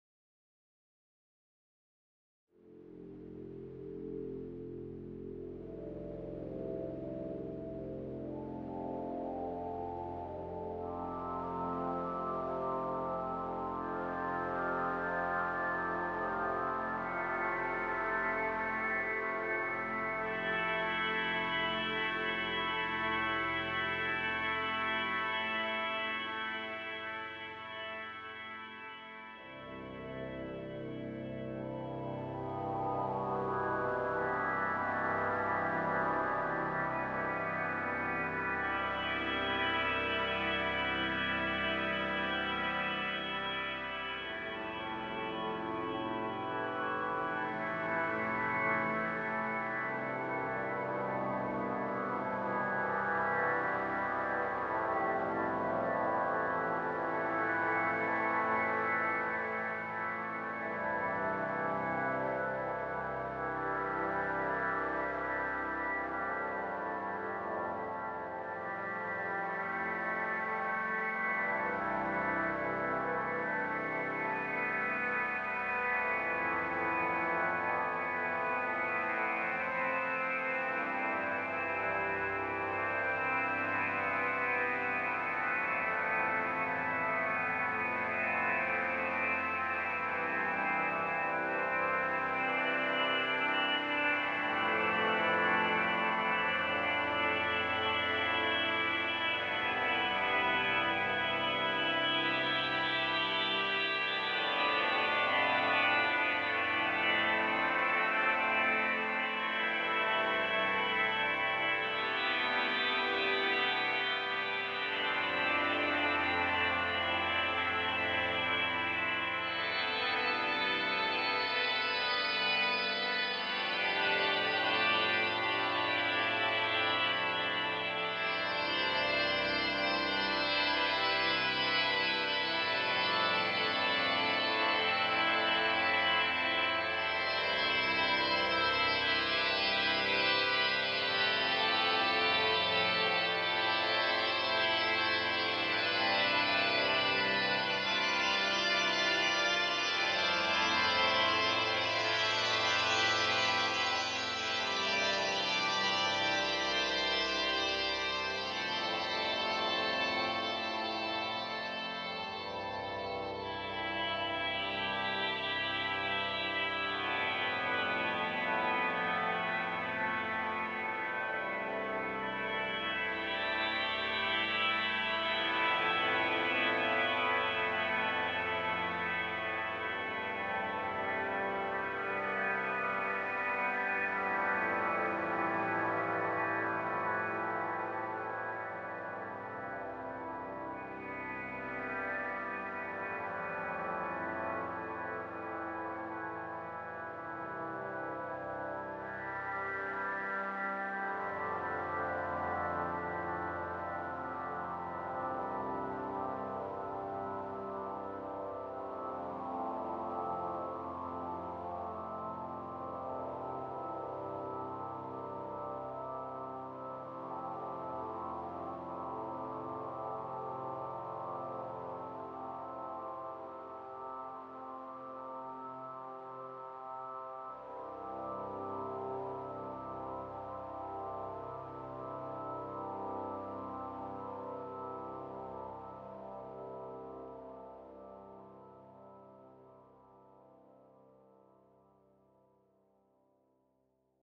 Sunrise | Ambient improvisation in SuperCollider
I made the audio using SuperCollider. I made the visuals using Processing. Unfortunately I didn't have any time to make something new this week, so I dug through older stuff and found something I really liked. (Actually, I stole the sun from this project for my logo.)

The synth I used in SuperCollider is a fairly basic one. I filtered white noise through a set of very resonant bandpass filters tuned to harmonics 1 through 4, making a sound like a faded organ. Then I hooked it up to my digital piano and improvised.

The sun and stars were made by brushing up on my high school trigonometry. The mountains / sound waves were made with the Minim library, something I don't fully understand. I suspected halfway through that I could have done it with Processing's native sound libraries, but Minim is too entrenched in my spaghetti code to change it.

The greatest issue I had with this was the process of recording the frames. Unfortunately the frame rate slowdown that results from saving frames to disk means that I can't properly record my audio-reactive visuals, at least not from within Processing. This time I got around that by saving the results of this audio reactivity to a table, which I then used during the recording session instead of the audio proper.

Please feel free to use this for meditation, or as a study aid, or as background music.